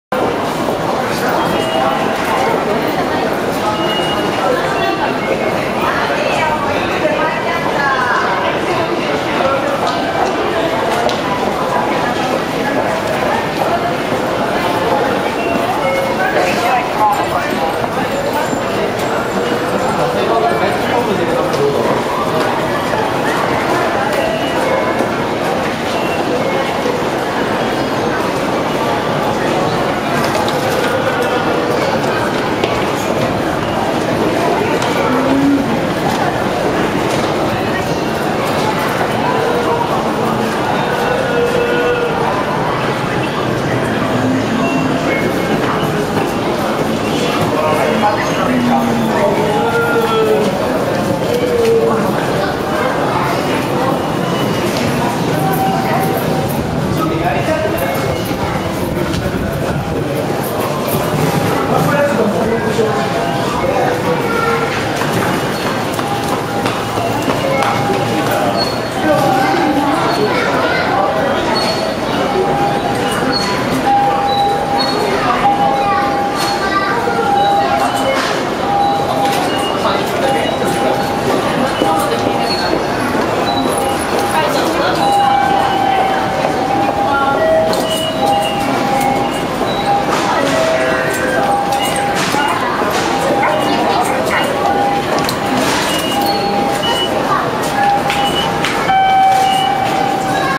2014.5 JR東日本と東京メトロの共同使用駅【中野駅】構内をクローズアップ・大きく取り上げてみる
前回の動画「JR東日本、中央・総武線「東中野駅→中野駅」（荻窪・三鷹方面）までの車窓風景【北側】 」
次回の動画「中野駅北口」→「中野サンモール商店街」→「中野ブロードウェイ」までの道のり

南口方面『もみじ山公園、中野図書館、中野保育所、中野警察署、中野消防署、なかのZERO』、階段、黄色い色の時計、化粧室、BECK'S COFFEE SHOP。KINOKUNIYA（パン屋）、精算機、成人式の広告、最後は改札口を映しました。